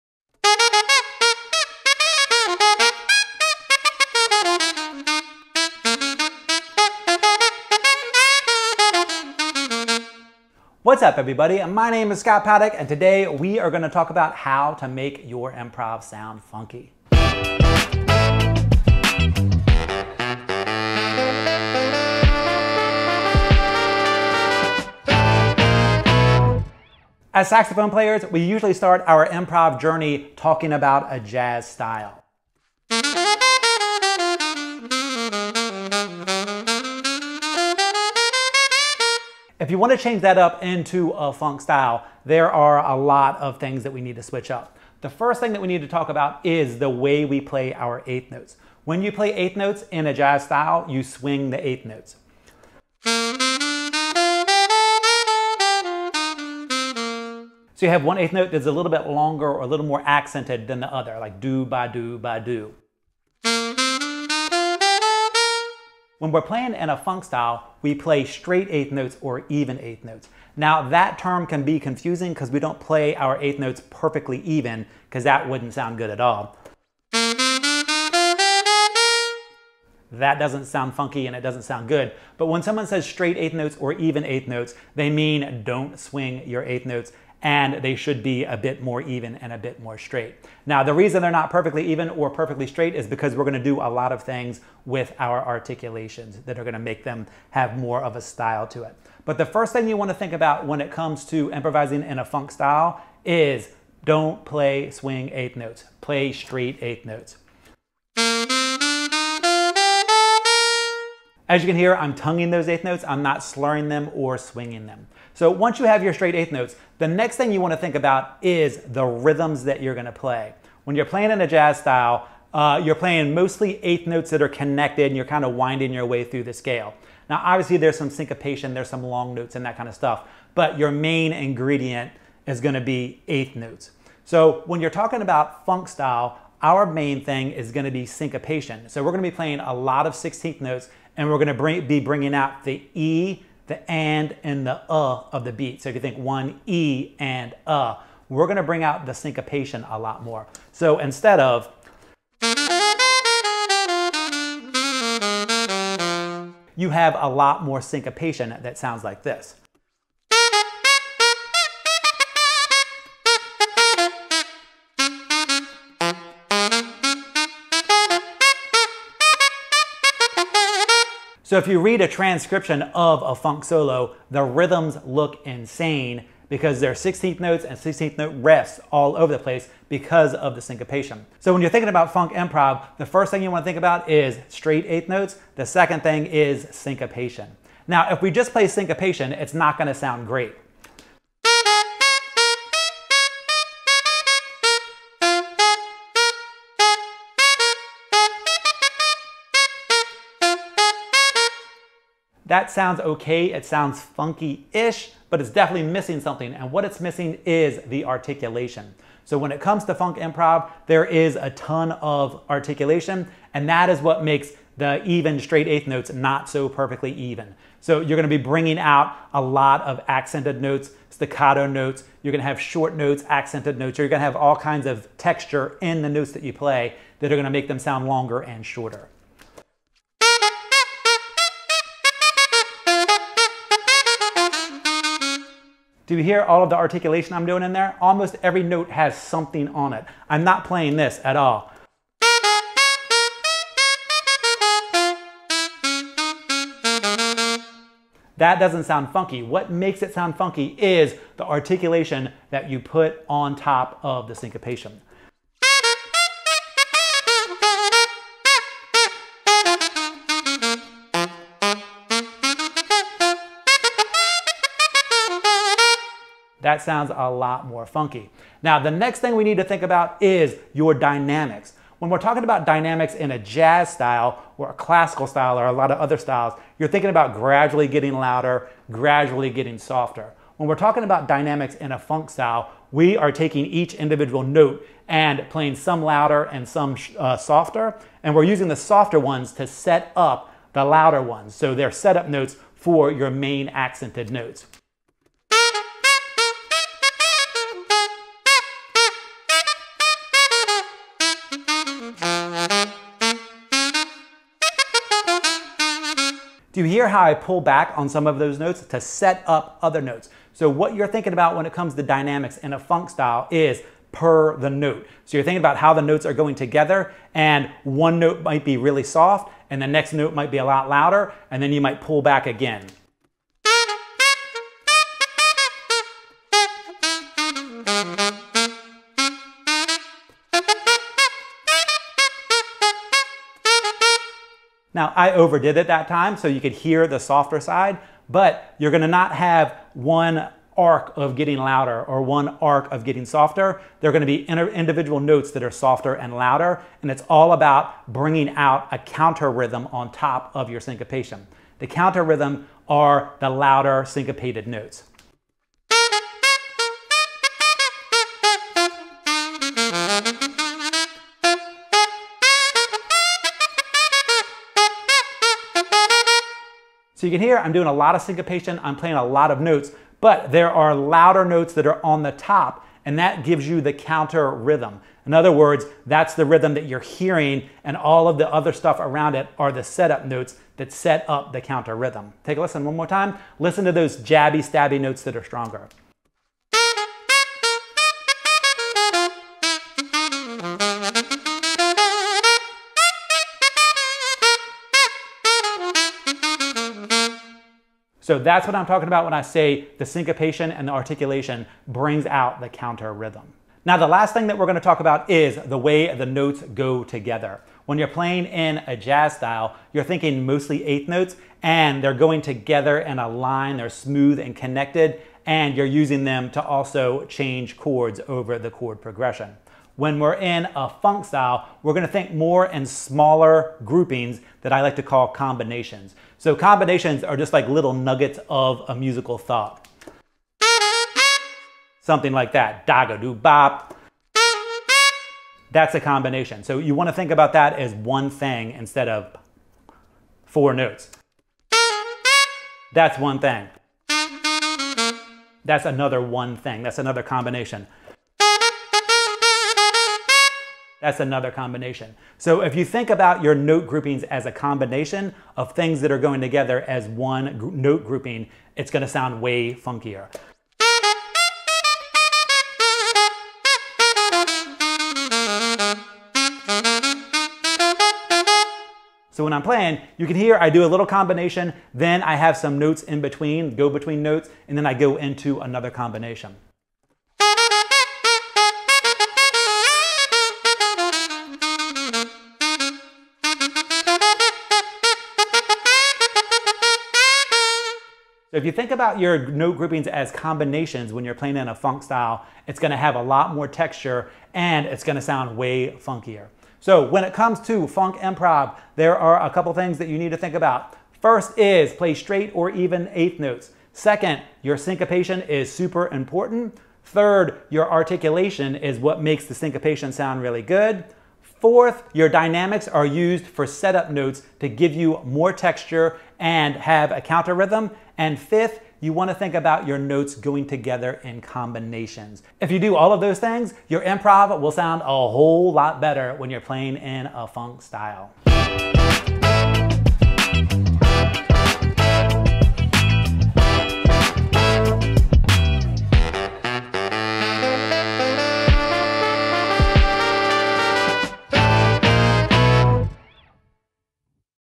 How To Make Your Improv Sound Funky with Scott Paddock
@ScottPaddock is back talking about improv funk soloing. Articulations, straight 1/8th notes, dynamics, syncopation and all sorts of tips that will make your playing funkier.